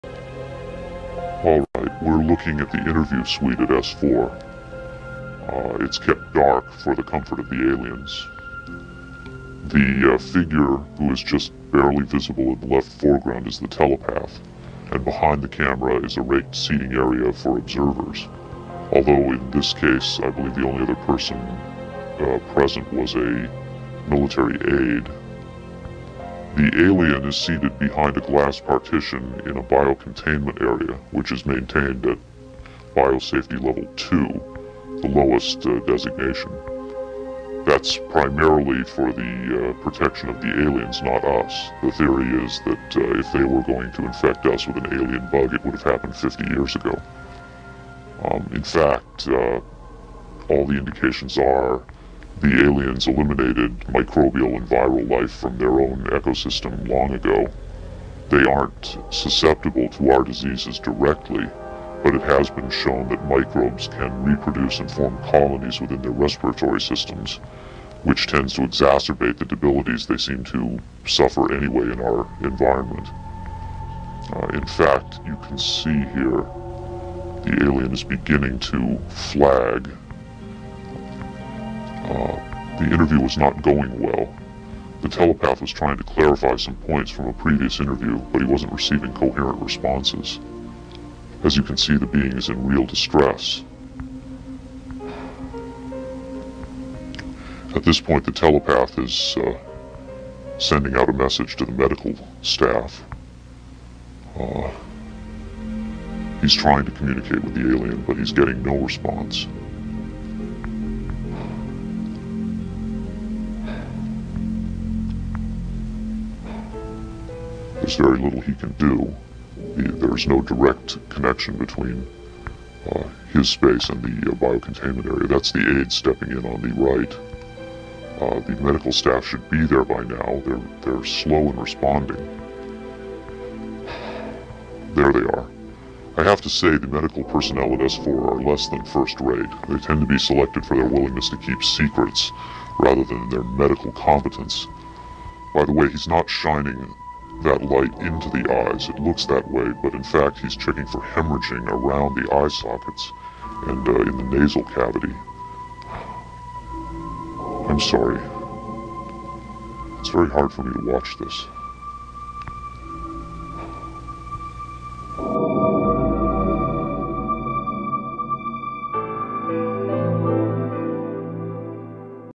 The Victor Alien Interview, w/Victor Narration
This is the only know video in existence of a real extraterrestrial.